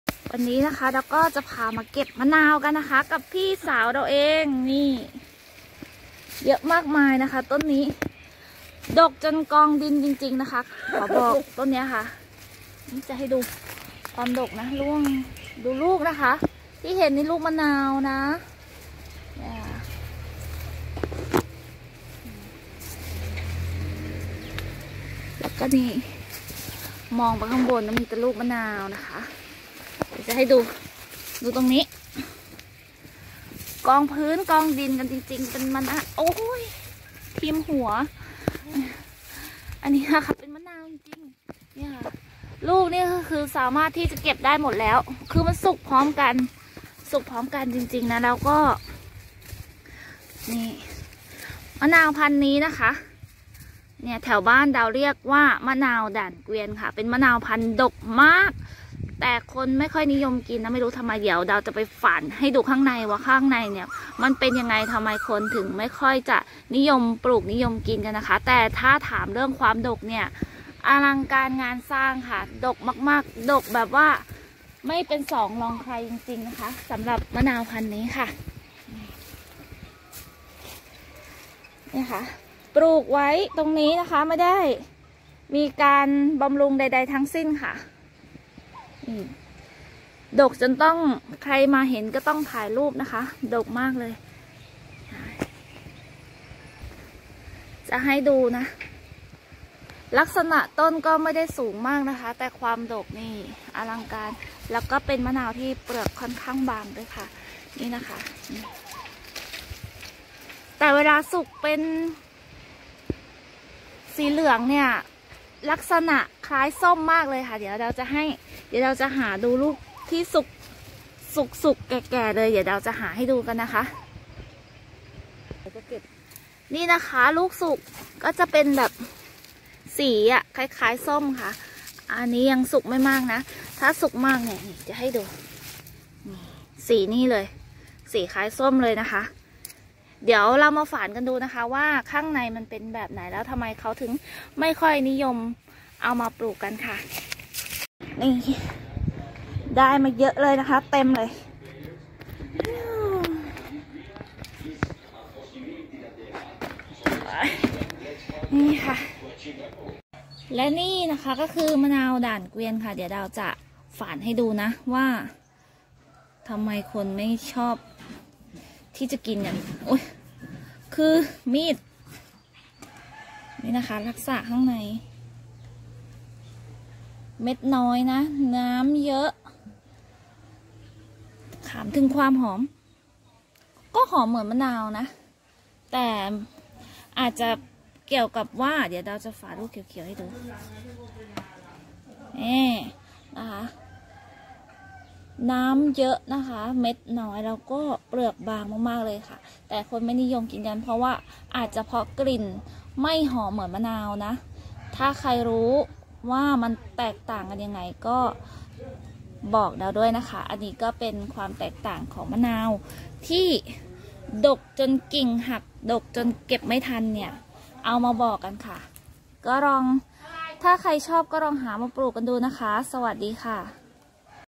แปลกจริง!!มะนาวพันธ์นี้ดกจนกิ่งหักเก็บไม่ทันปลูกง่าย แต่คนไม่นิยมปลูกไม่นิยมกินเพราะอะไร
วันนี้พาดูมะนาวดกๆ เลี้ยงง่ายแต่ไม่ค่อยมีใครกินกันจร้า